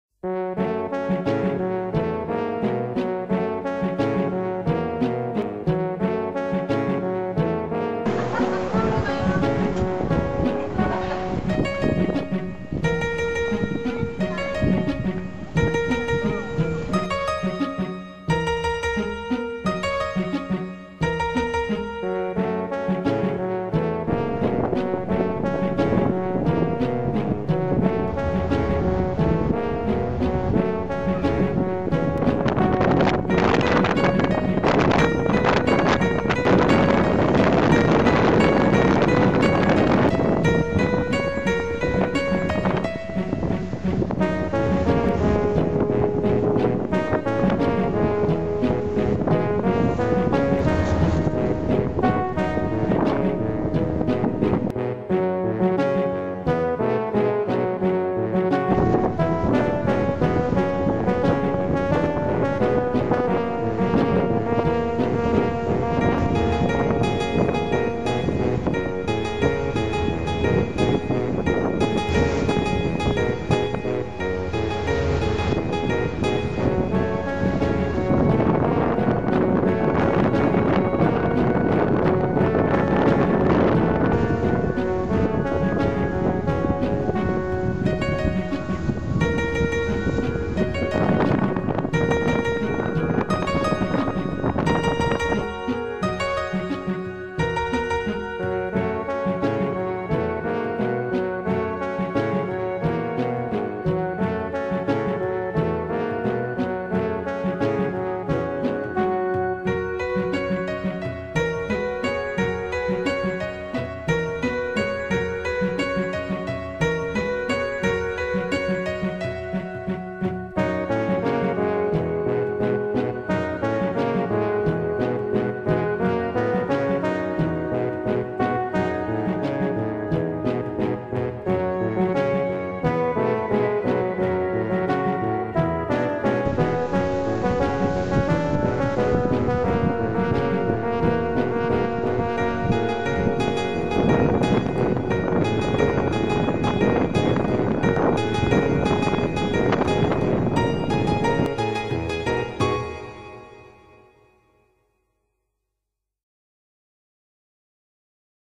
佐田岬（日本秘境百選）と国道九四フェリ－.wmv
九州と四国を結ぶ最短コ－スの国道九四フェリ－、７０分で佐賀関～三崎を結ぶ。全長４０ｋｍの佐田岬半島の岬は四国最西端であり日本秘境百選でもある。